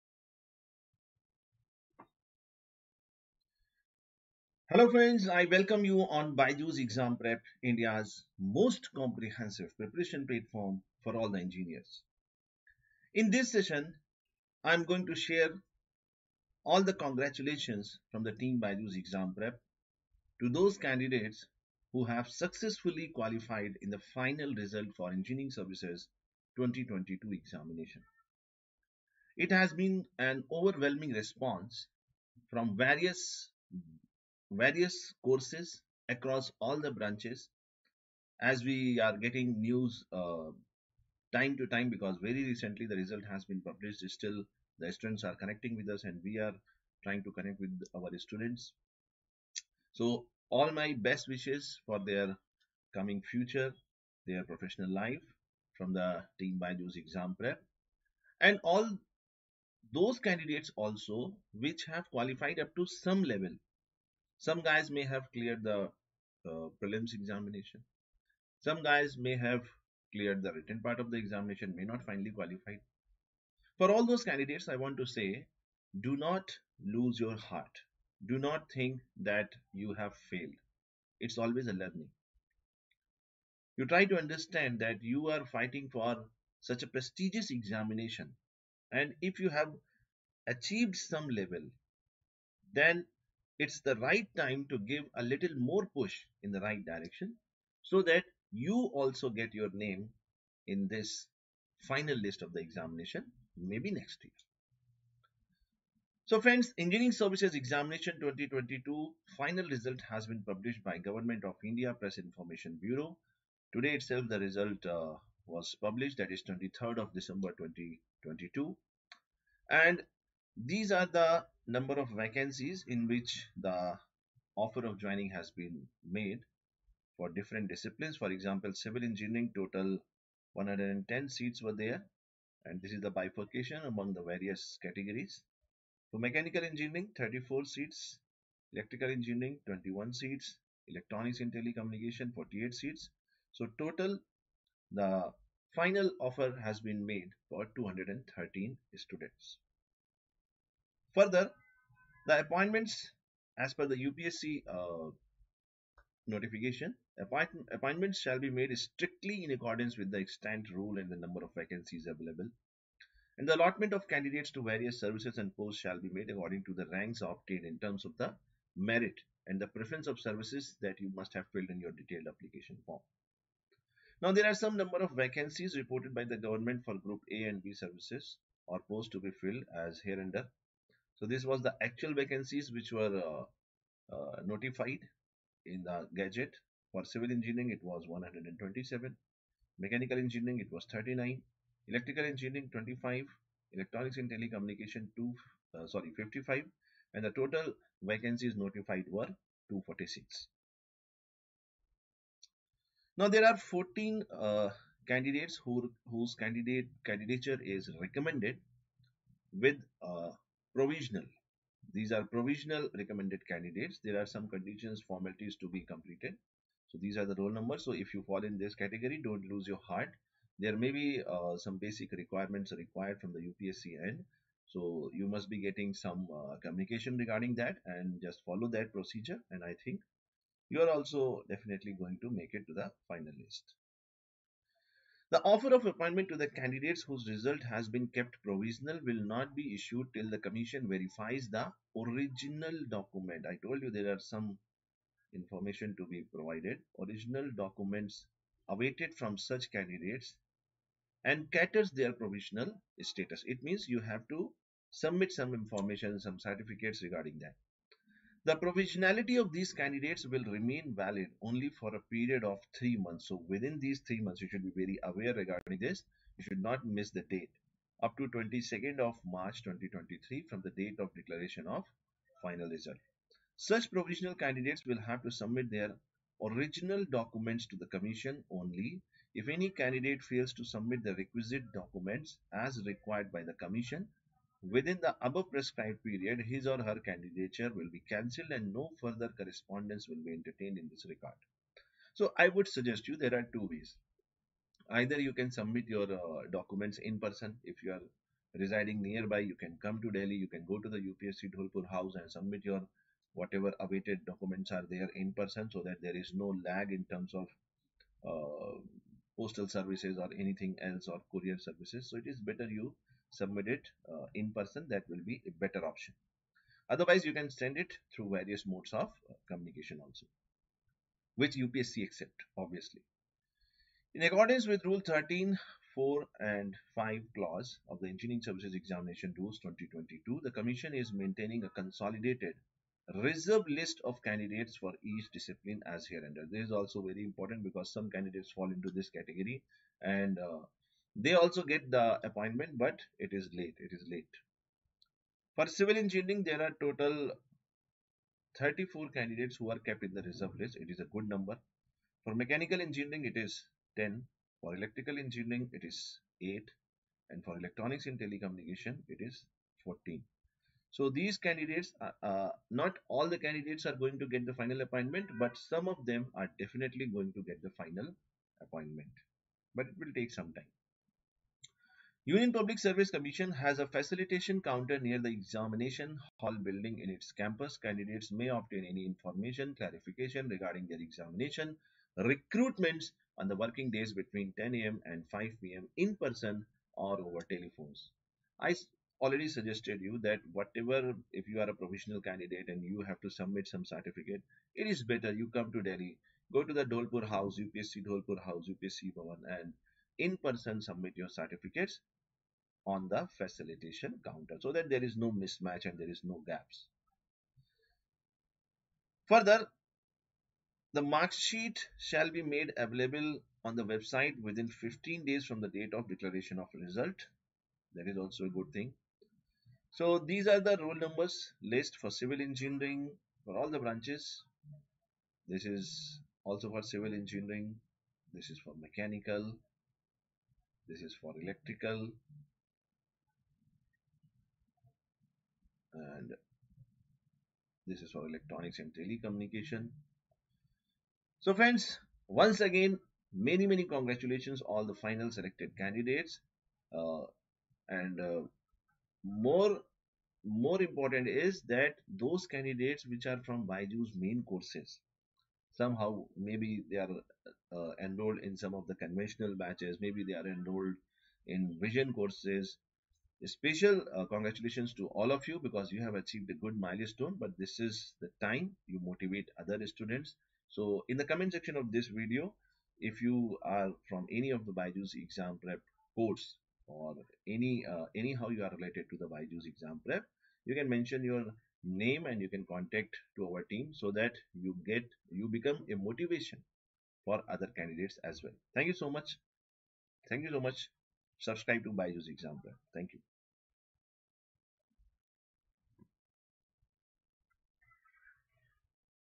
ESE 2022 Result | ESE 2022 Final Result Out 👨‍🎓| ESE 2022 Result Complete Information
The ESE 2022 results are finally out! Get ESE 2022 Result complete information about the ESE 2022 result here.

In this session, BYJU'S Exam Prep GATE expert, Ashutosh Sir, takes you through the complete details of the UPSC Engineering Services 2022 result. The UPSC ESE is one of India's most prestigious competitive exams for graduate engineers. The exam is conducted in three levels: Prelims, Mains and Interviews. The ESE result combines the marks of these three steps and releases the final list of recommended candidates based on it. In 2022, 11.52 lakh candidates applied, out of which 13090 cleared the prelims, and finally, 213 students got recommended in the IES result 2022.

The main highlights of this session are:
- Important points about the UPSC ESE result 2022
- Press release of ESE 2022 result
- Recommended candidates in each stream
- Total vacancies for ESE 2022
- Candidates in ESE 2022 Reserve list

Congratulations to all the candidates who finally cleared the ESE 2022 exam. The IES result 2022 is a motivation for all students preparing for ESE 2023. Tune into this session to get all the important information about the UPSC ESE result 2022.

Share this ESE 2022 result session with your engineering friends preparing for the ESE exam to motivate them.